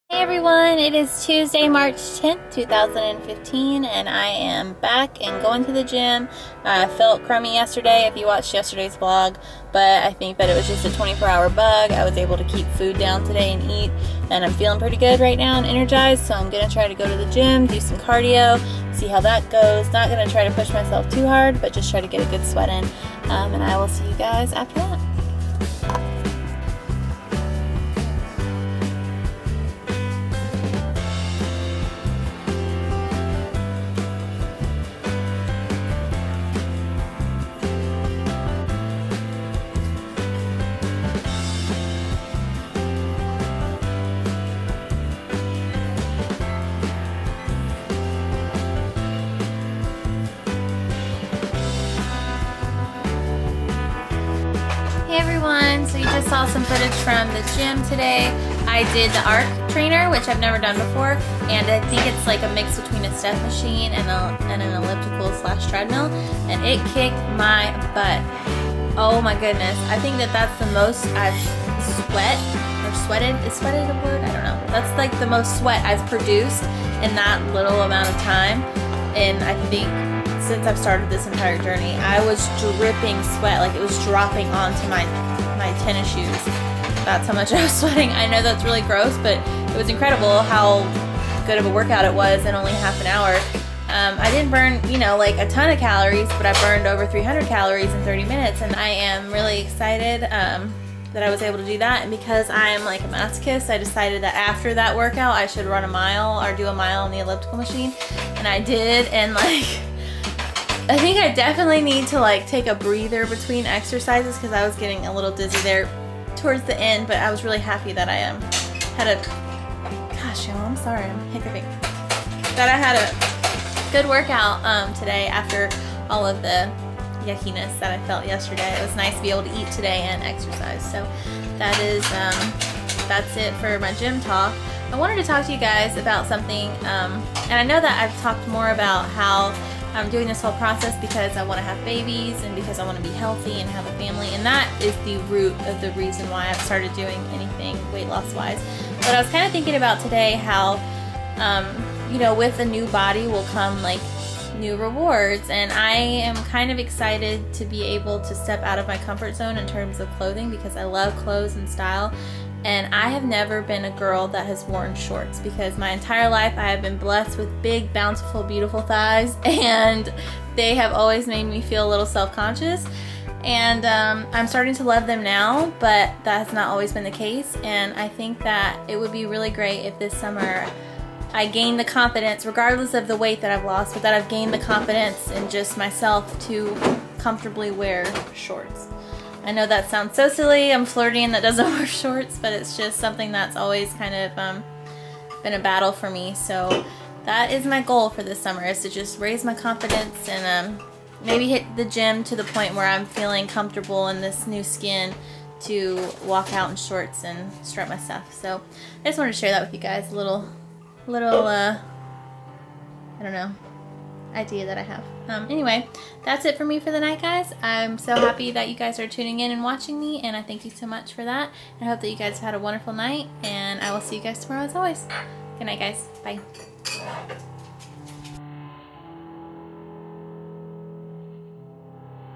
3/10/15 Day 46
Day 46! Back to the gym today, and I share an insecurity of mine. Tomorrow I film with Emotional Mojo, so i'll link a teaser of that up tomorrow!